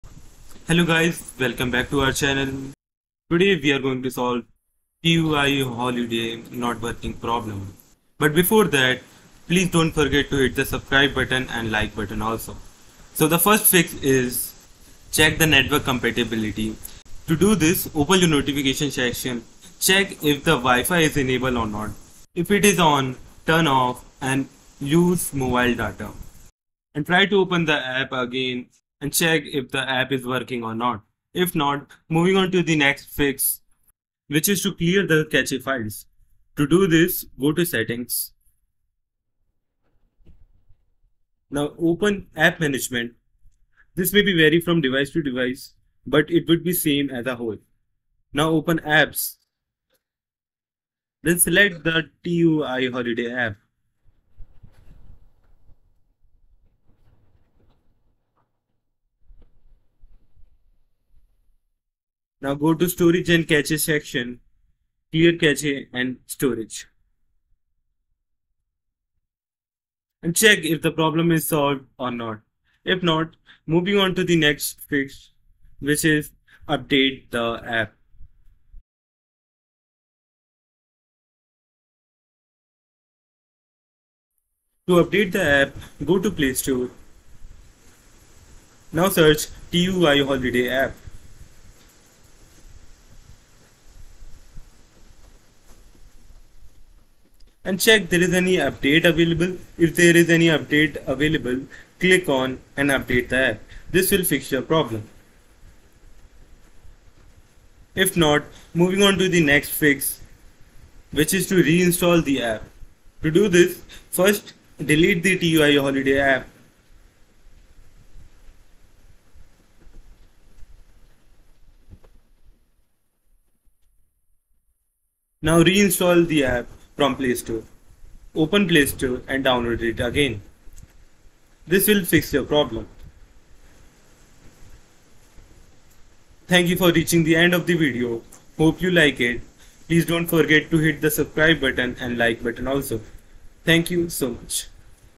How to Fix TUI Travel App Not Working, Keep Crashing, Keep Stopping, Stuck on Loading Screen
In this straightforward tutorial, we'll walk you through the steps to resolve the issue of the Tui Holiday app not opening on your device. Follow along as we provide clear and concise instructions to get your app up and running smoothly in no time. Say goodbye to the frustration and let's get started!

0:00 - Intro
0:16- Fix 1: Check your network connection
0:34 - Fix 2: Clear TUI Travel App Cache file and storage
1:15 - Fix 3: Check TUI Travel App Updates
1:50 - Fix 4: Uninstall & Reinstall the TUI Travel App